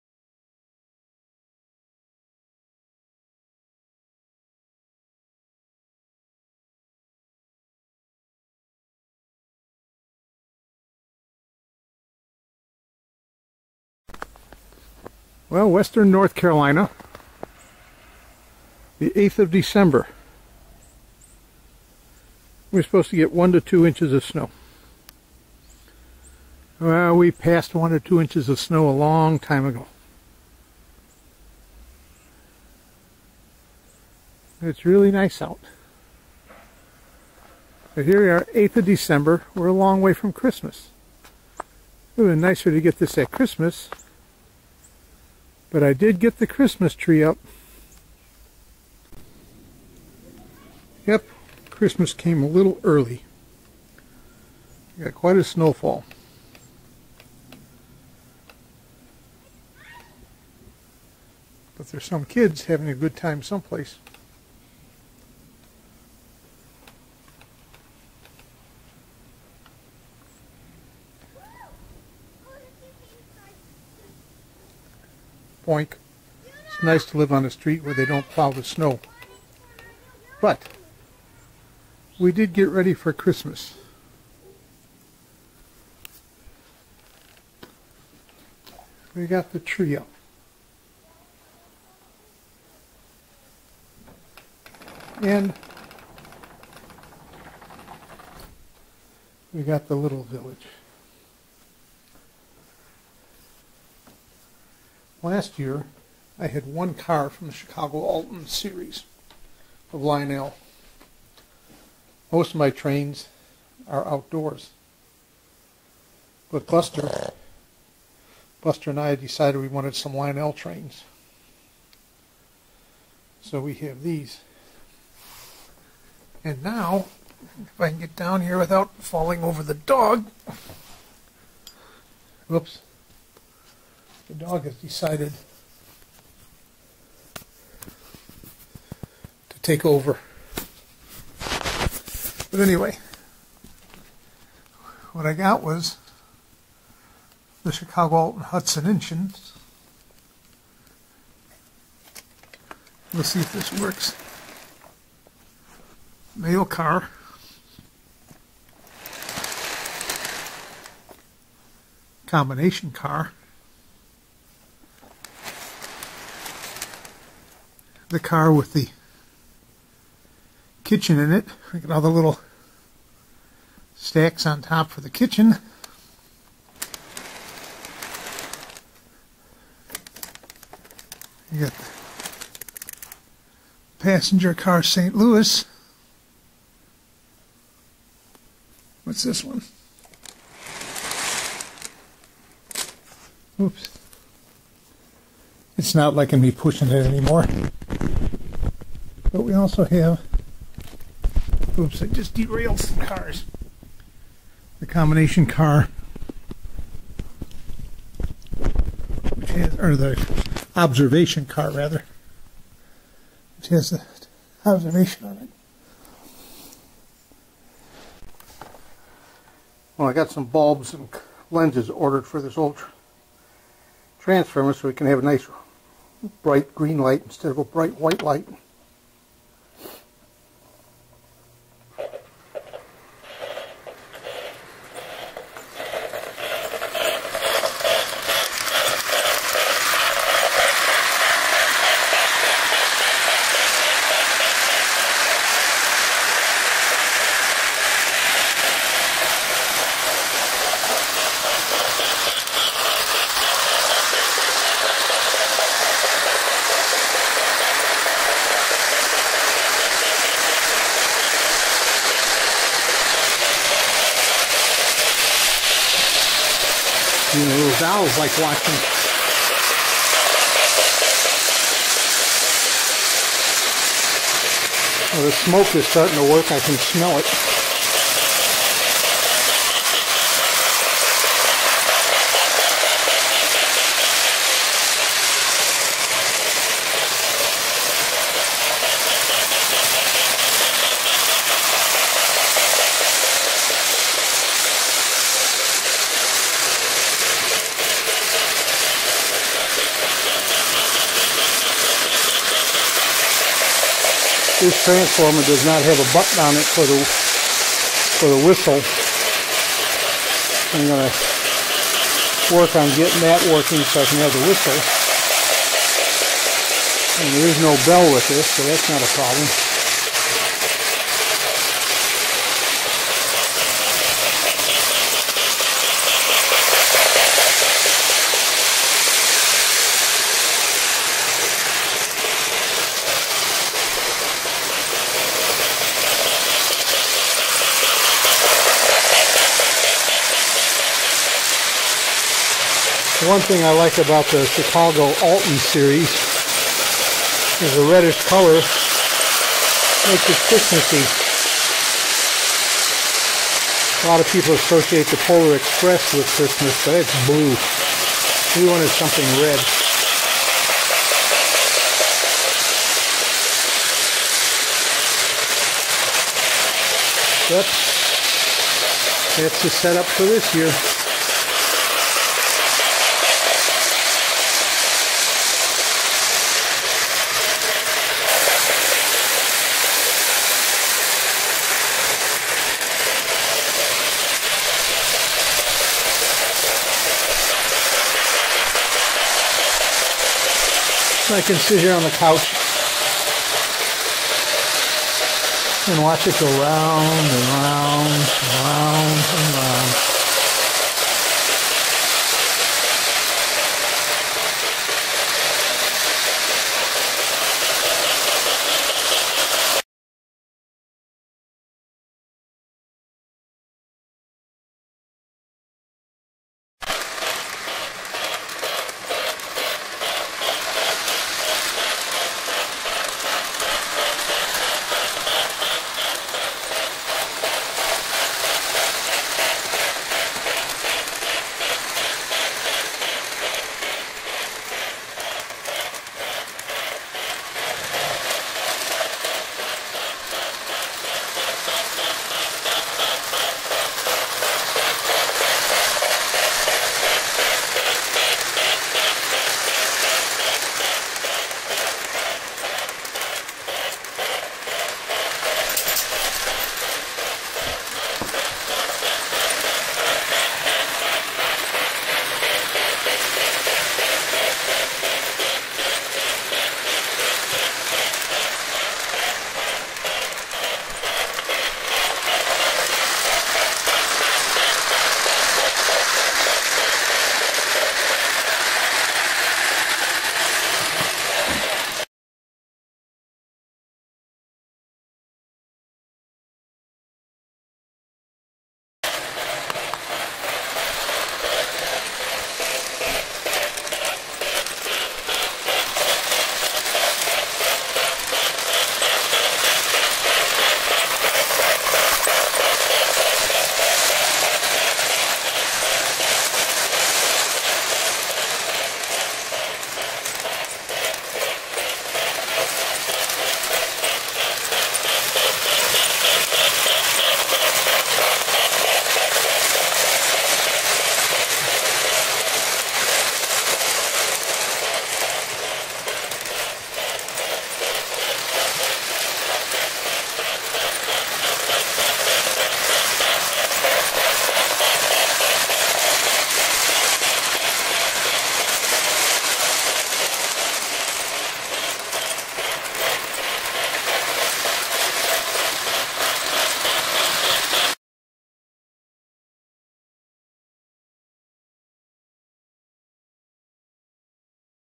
Lionel Chicago Alton Christmas 2017
Lionel Chicago Alton set. Hudson engine and six cars. series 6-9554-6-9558. and the later diner car 6-9599. On Fastrack with an old type V transformer.

I only run Lionel at Christmas. I have an outdoor G scale - Hotdog Garden Railroad